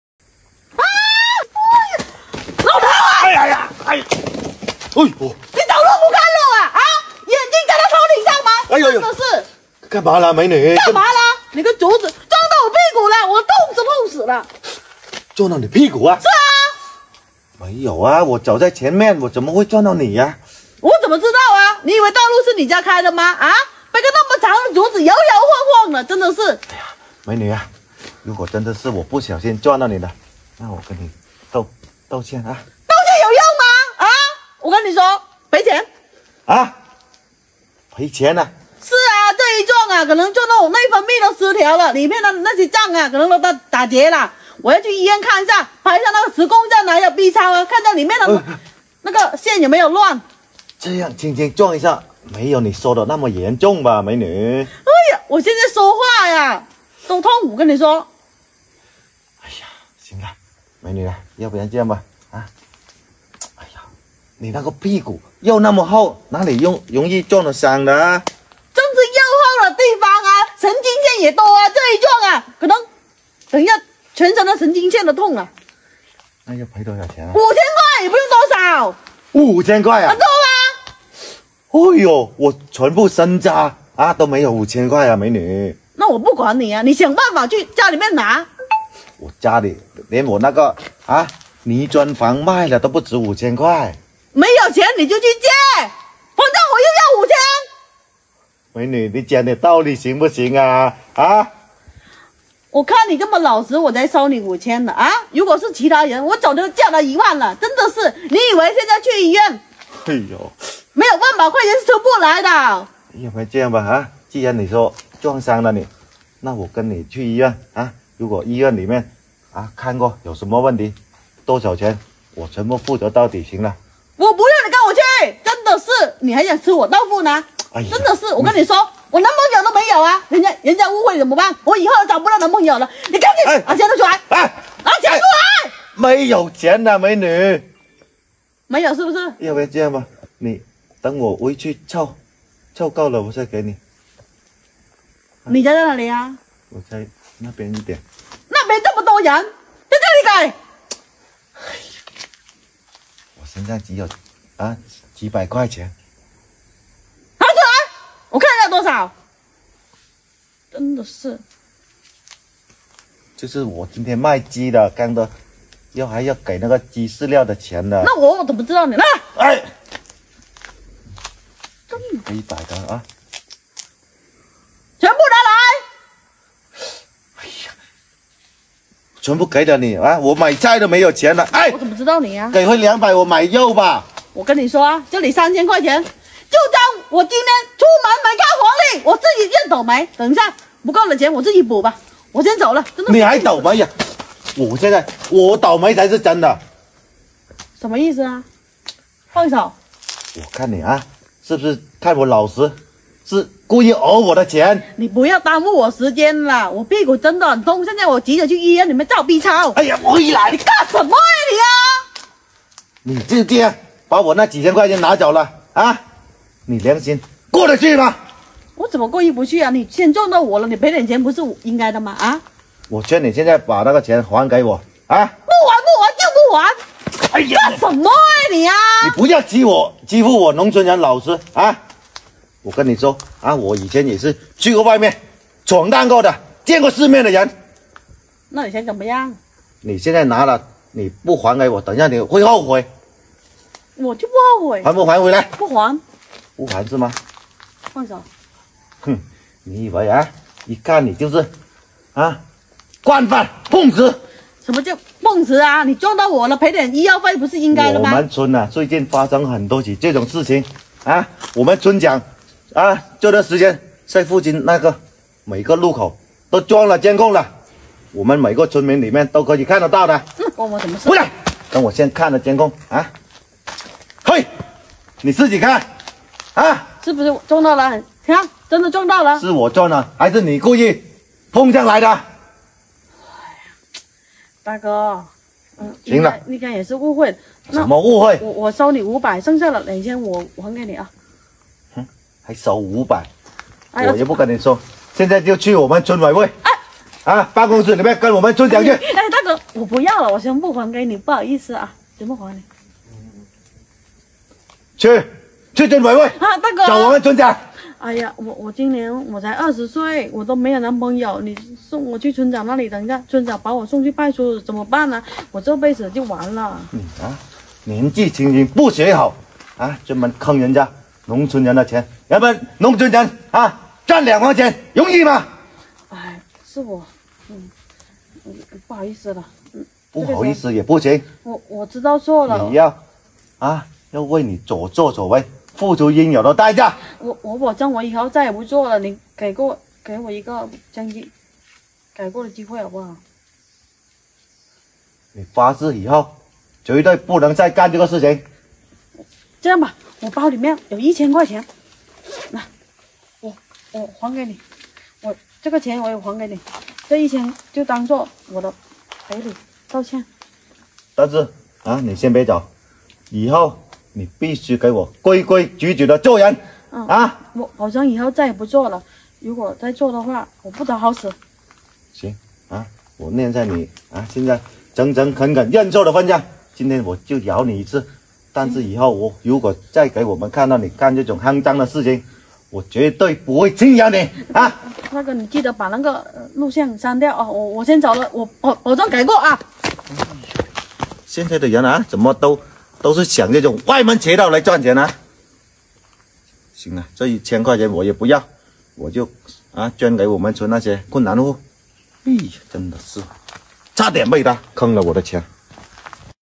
美女半路碰瓷農村小夥，張口就要5000元，誰料小夥一招讓她損失1000元！ 【李石瀾】
哈嘍哈嘍！大家好！
我是李石瀾👧
用心演好每個角色 💖

鄉村短劇 请放心，本视频是我们精心制作的原创短剧，没有任何不良行为。